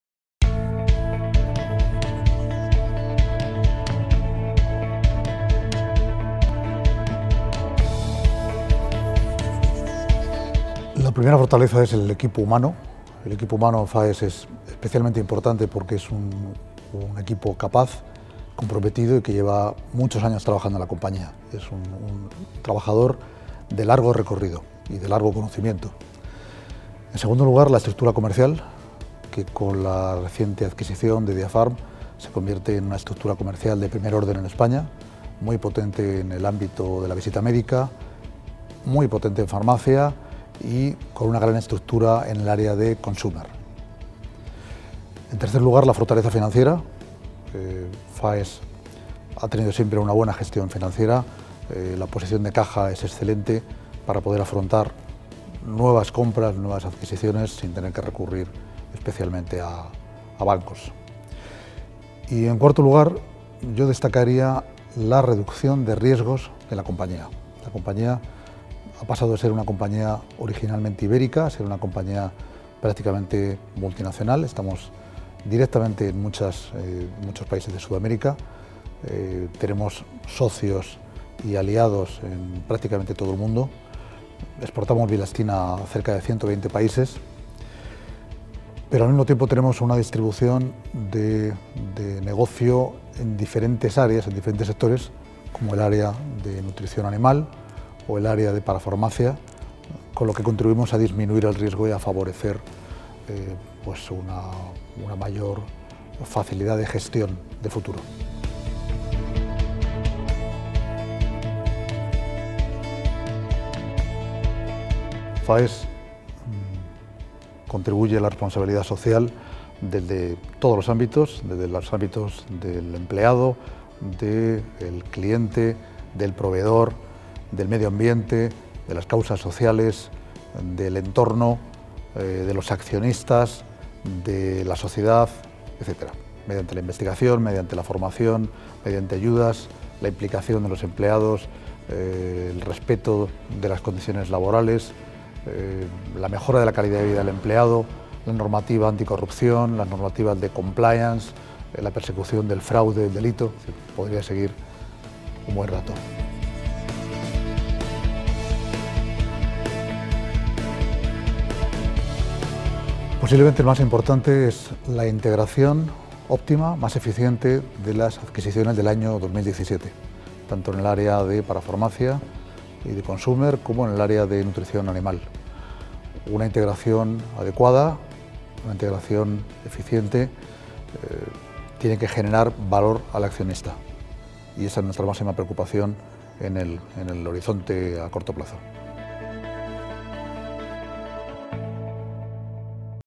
Francisco Quintanilla, General Manager de Faes Farma (Foro Medcap 2018)
Entrevista a Francisco Quintanilla, General Manager de Faes Farma, realizada durante la decimocuarta edición del Foro MedCap (29, 30 y 31 de mayo de 2018). El Foro Medcap es el evento de referencia y cita imprescindible para inversores europeos y empresas de mediana y pequeña capitalización cotizadas en España.